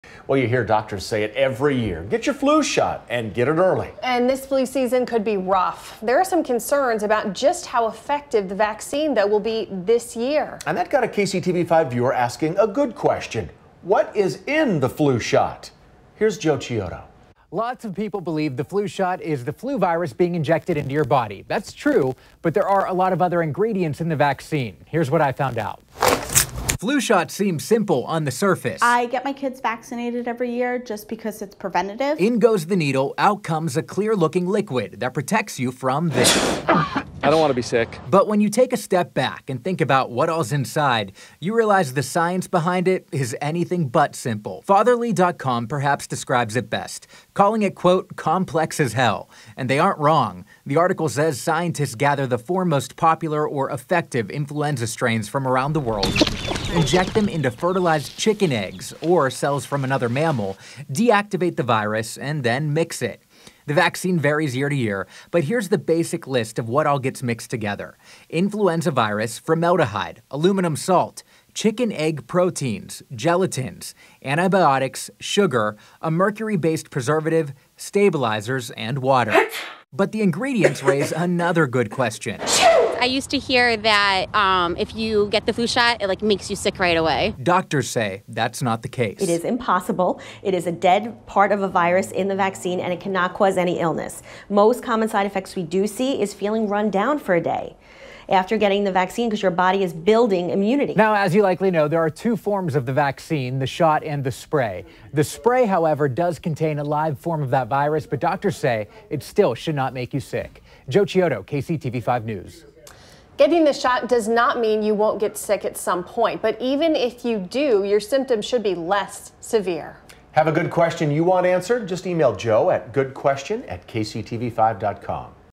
Good Question: What is in the flu shot?
You hear doctors say it every year, get your flu shot and get it early. This flu season could be rough. There are some concerns about just how effective the vaccine will be this year.